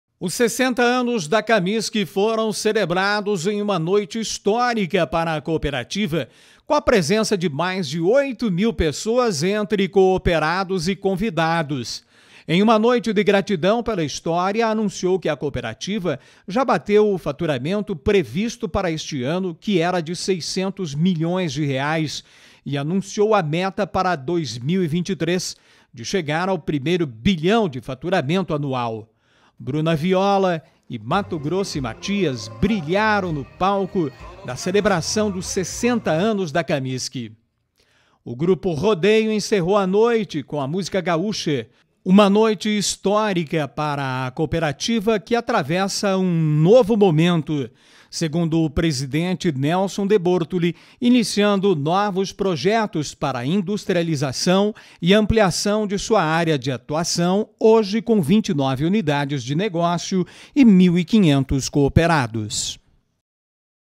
60 anos da CAMISC
Atualidade: Shows Nacionais marcaram o aniversário de 60 anos da CAMISC.